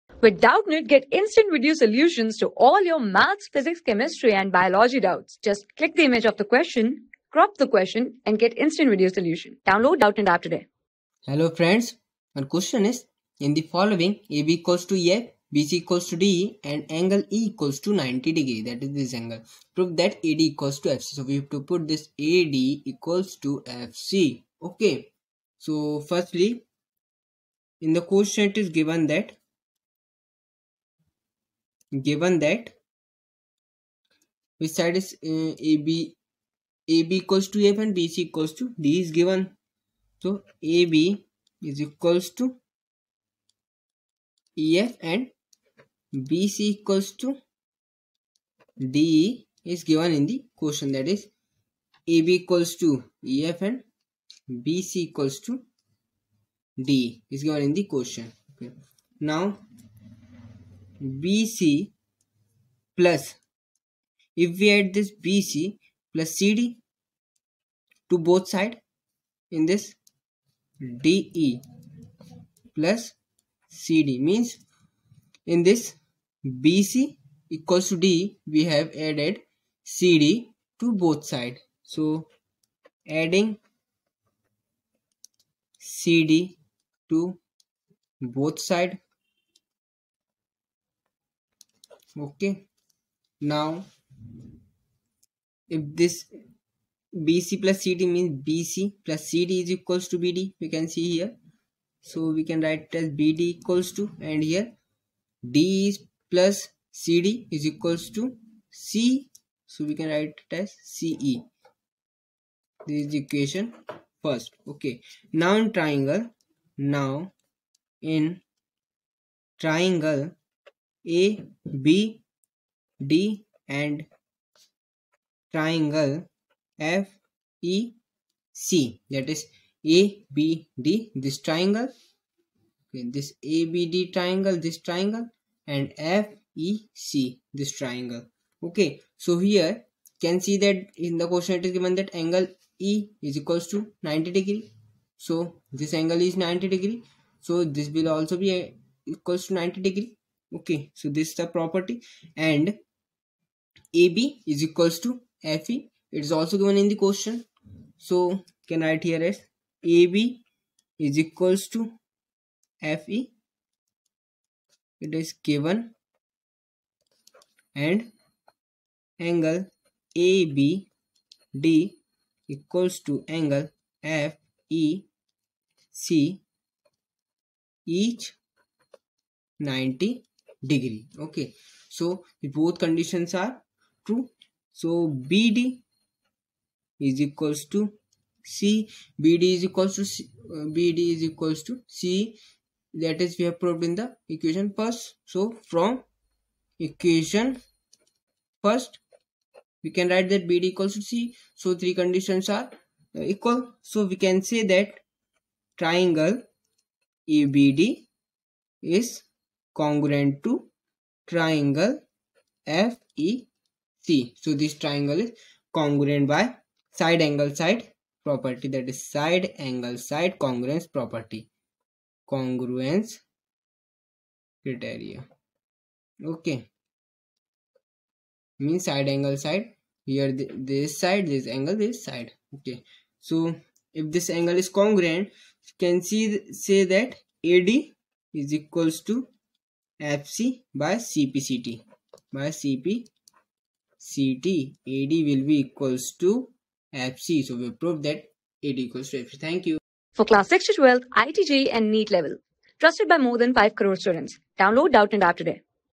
In the following, AB=EF, BC=De and angleE=90^(@) Prove that AD=FC | 9 | TRIANGLES | MATHS | ICS...
In the following, AB=EF, BC=De and angleE=90^(@) Prove that AD=FC
Class: 9
Subject: MATHS
Chapter: TRIANGLES
Board:ICSE

👉 Have Any Query? Ask Us.